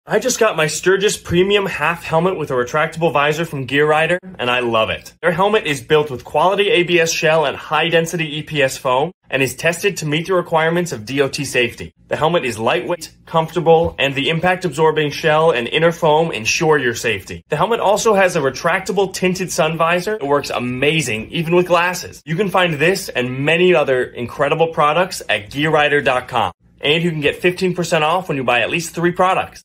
Sturgis Premium Half Helmet - Retractable Visor
DOT CERTIFIED HELMET

Our Half Helmet is built with quality ABS shell and high-density EPS foam, it is tested and meets the requirements of DOT safety standards. The half helmet is lightweight and comfortable, the impact-absorbing outer shell and inner foam ensure your safety while riding a motorcycle.

Unique Features:

High Impact ABS Shell provides for Great Energy Diffusion.
Quick Release Chin Strap Easily Get It On and Off
Quality Improved Integrated Drop Down Sun Visor, Removable and Replaceable
Premium Venting Reduces Heat at The Top of The Head

Standard Features:
Meets or exceeds D.O.T. FMVSS No.218 standards & AS/NZS 1698
1" Thick Polystyrene Protection Layer meets the Minimum Requirement for DOT Certification

Fiberglass Shell with a High Impact for Superior Energy Diffusion

Quick Release Buckle
You can easily take on and off the motorcycle half helmet with quick release buckle.

Removable Ear Flap

The ear flap is comfortable and skin-friendly.
You won’t feel squeezing around face when adjusting the tightness of the helmet with chin straps.

Retractable Visors
The half helmet is assembled with tinted sun visor which is easy to retract if you don't want to use the visor.

How to correctly measure your head: take a tape measure just above your eyebrow around your head just at the top of your ears. If your head measures in between sizes order the size smaller. Our helmets should fit snug when new, they stretch and mold to the shape of your head after short usage.

Smallest Lightest DOT Open Face Helmet